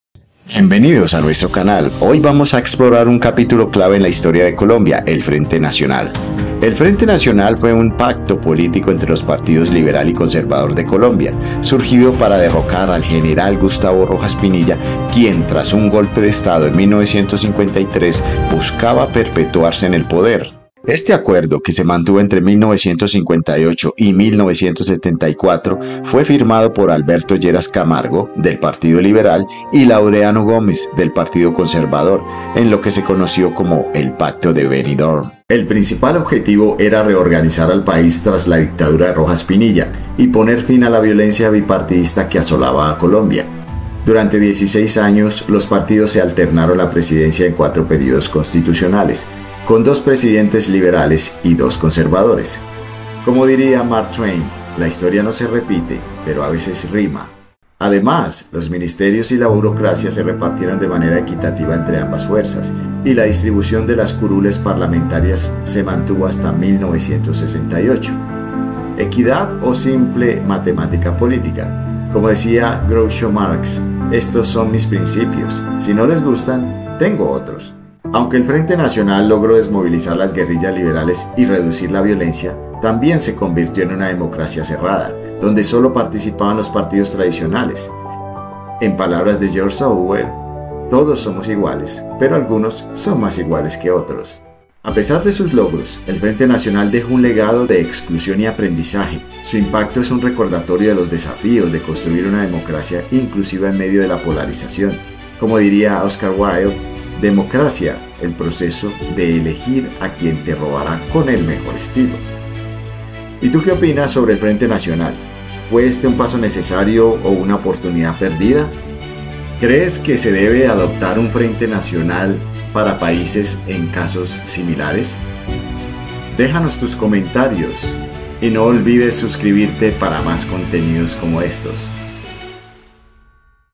El acuerdo histórico entre liberales y conservadores
Explora el Frente Nacional, el acuerdo histórico entre liberales y conservadores que reorganizó a Colombia tras una dictadura. Descubre cómo este pacto buscó poner fin a la violencia bipartidista, dando lugar a 16 años de poder compartido. ¿Fue la solución que necesitaba el país o un paso hacia nuevos conflictos? Sumérgete en esta fascinante historia de alianzas y decisiones que marcaron la política colombiana.